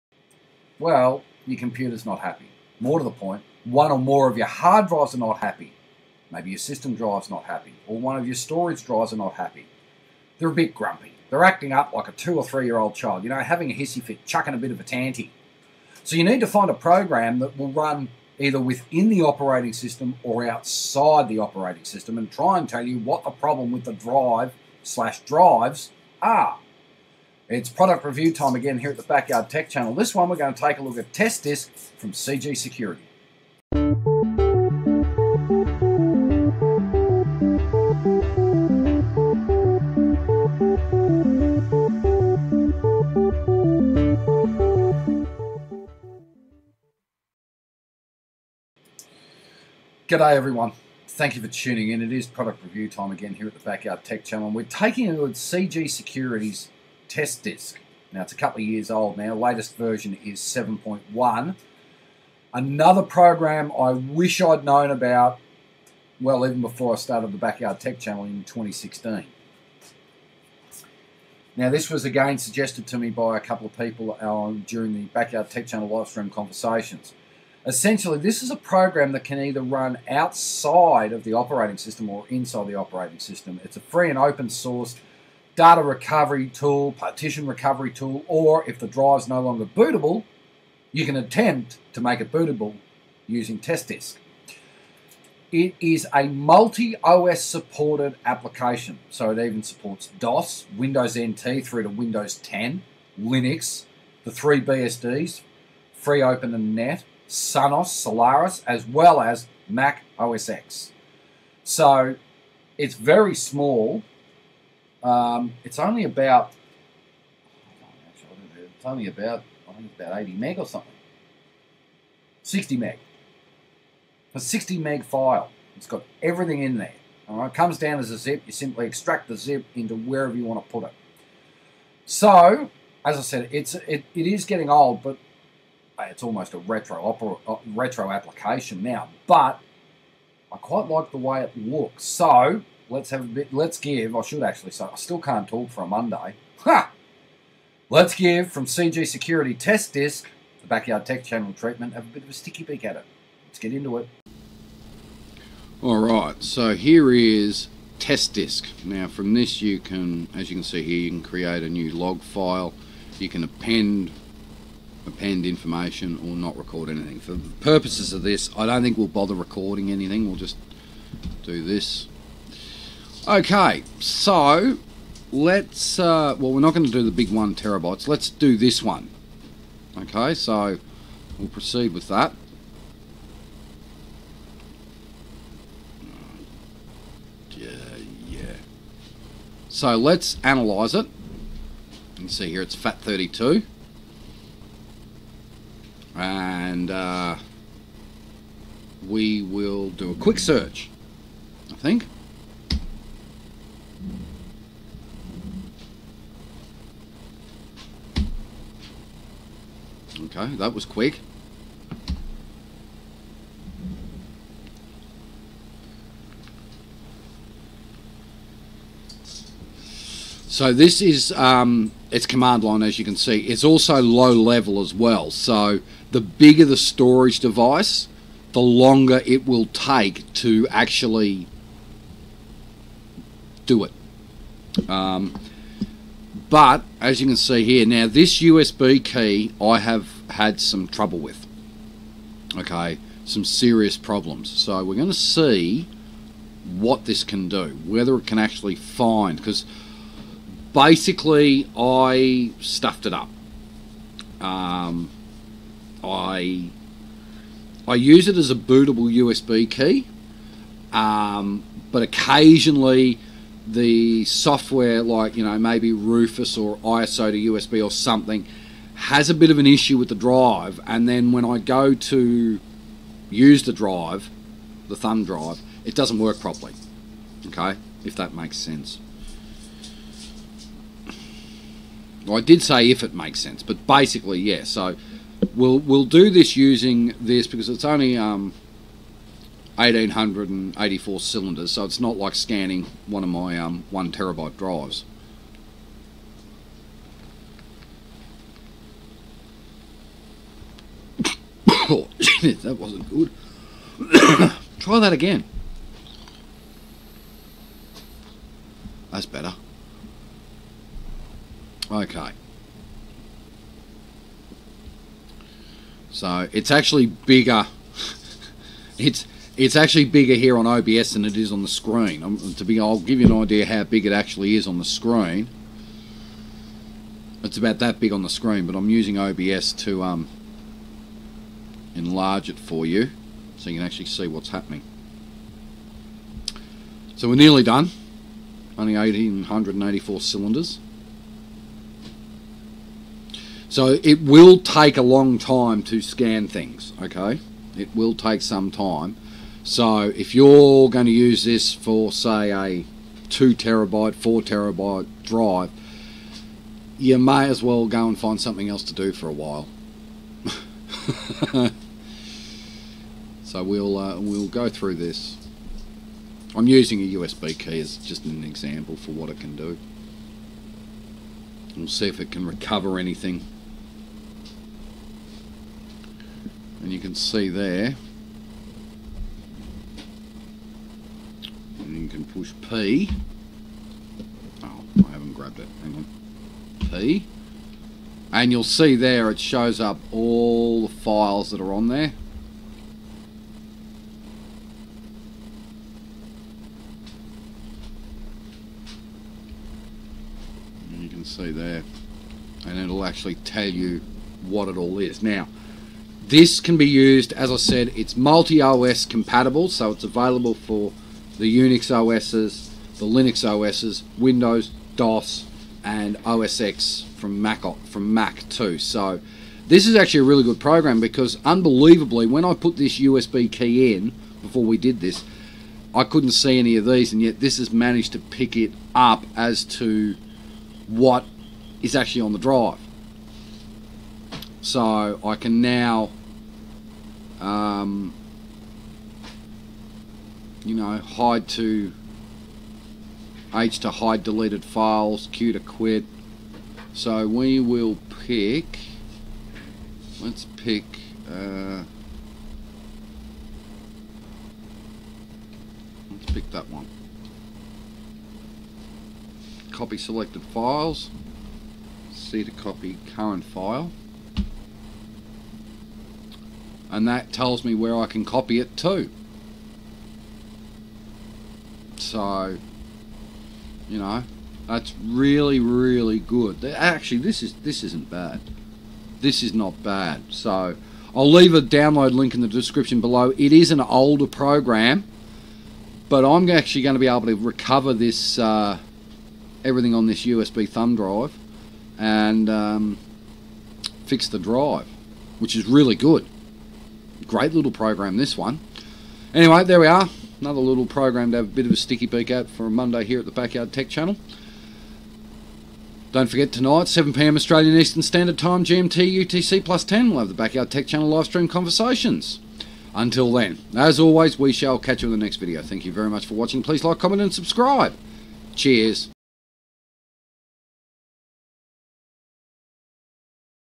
Product Review - TestDisk
In this video, I take a look TestDisk from CG Security